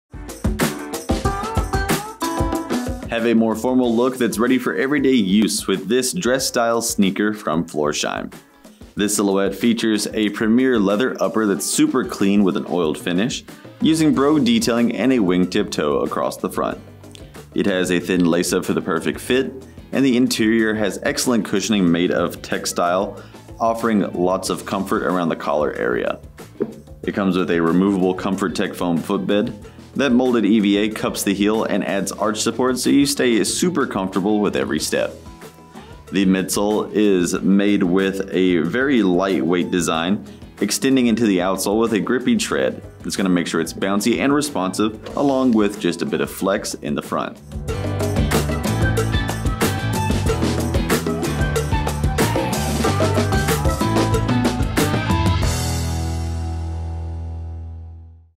Florsheim Premier Wing Tip Lace-Up Sneaker SKU: 9709513
Stay stylish in comfort with the Florsheim&reg; Premier Wing Tip Lace-Up Sneaker.
Leather upper.
Lace-up closure.
Round-toe silhouette.
Textile lining and insole.
Man-made outsole.
Imported.